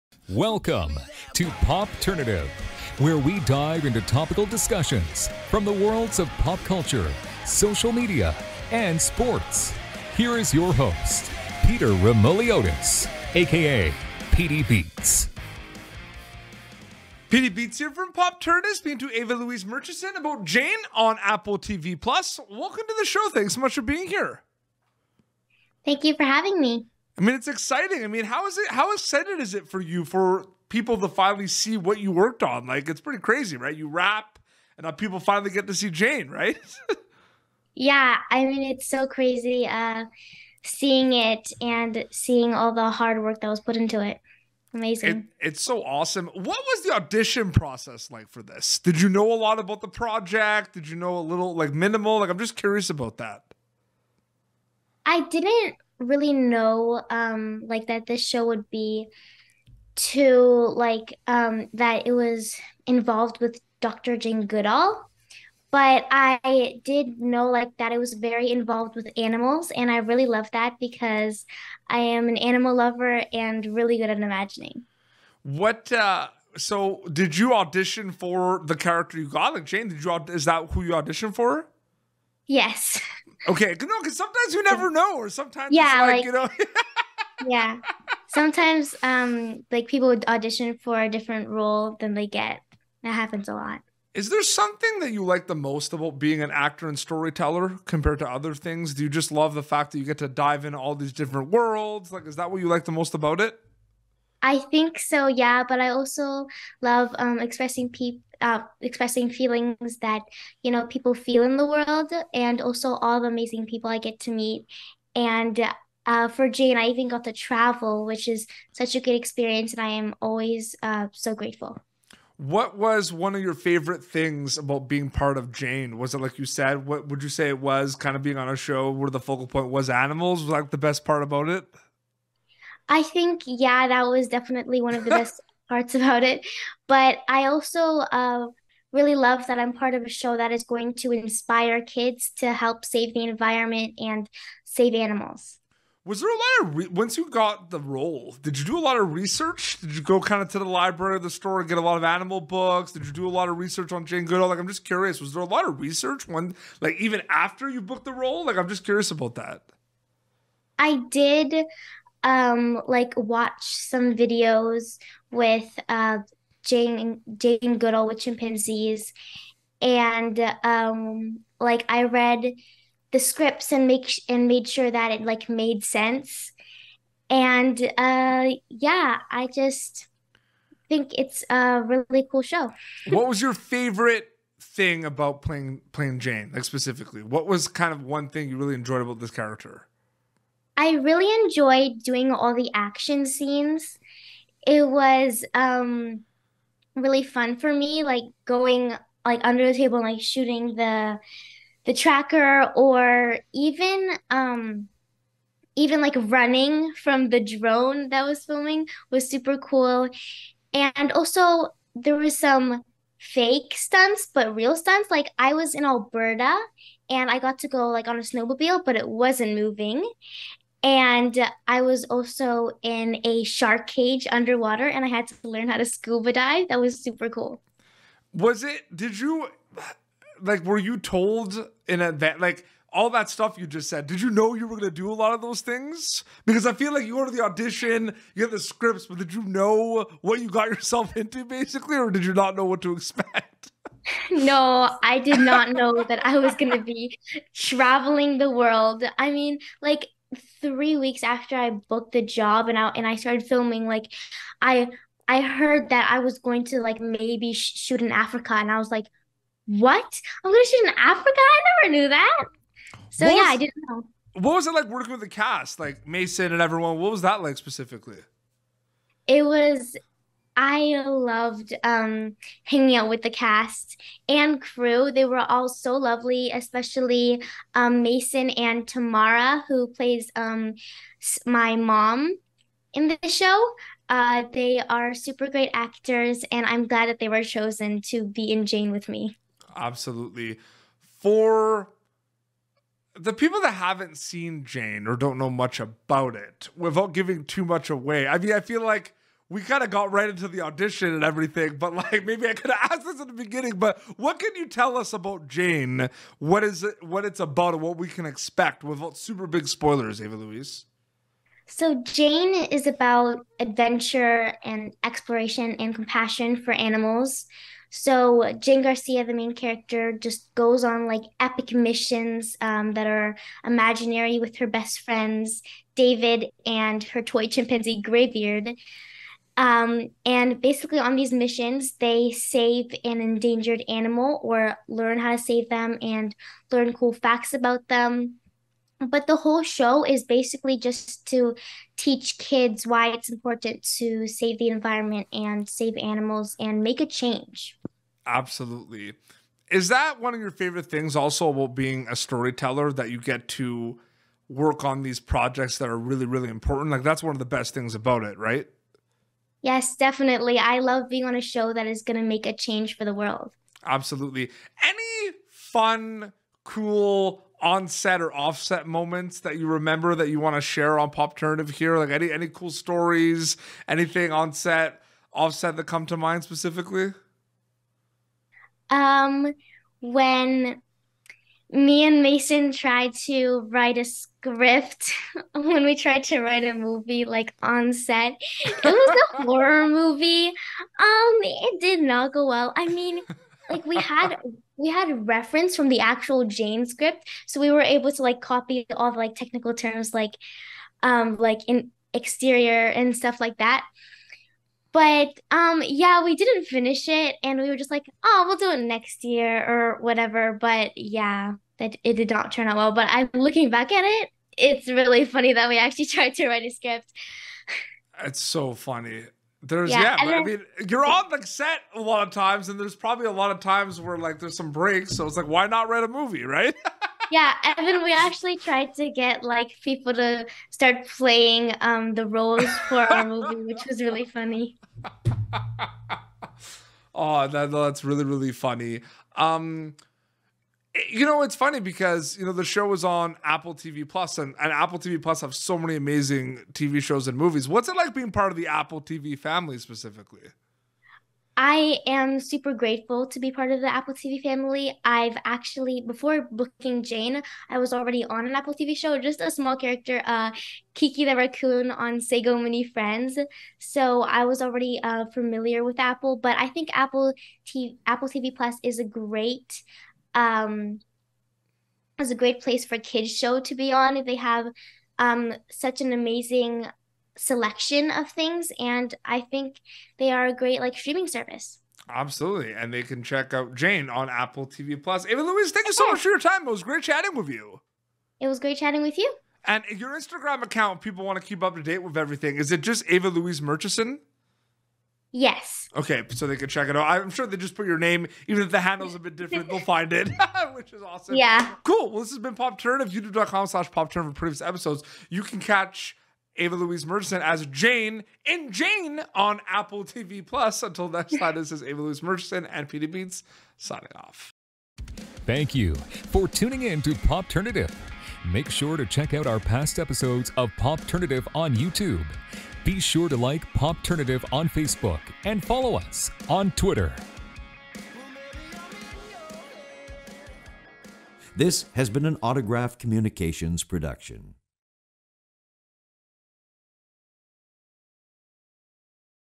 Ava Louise Murchison talks about Jane on AppleTV+ and much more!
Jane, a 9-year-old budding environmentalist on a quest to save endangered animals. Using her powerful imagination, Jane takes her best friends David and Greybeard the chimpanzee on epic adventures to help protect wild animals all around the world because, according to her idol Dr. Jane Goodall: “Only if we understand, will we care. Only if we care, will we help. Only if we help, can they be saved.”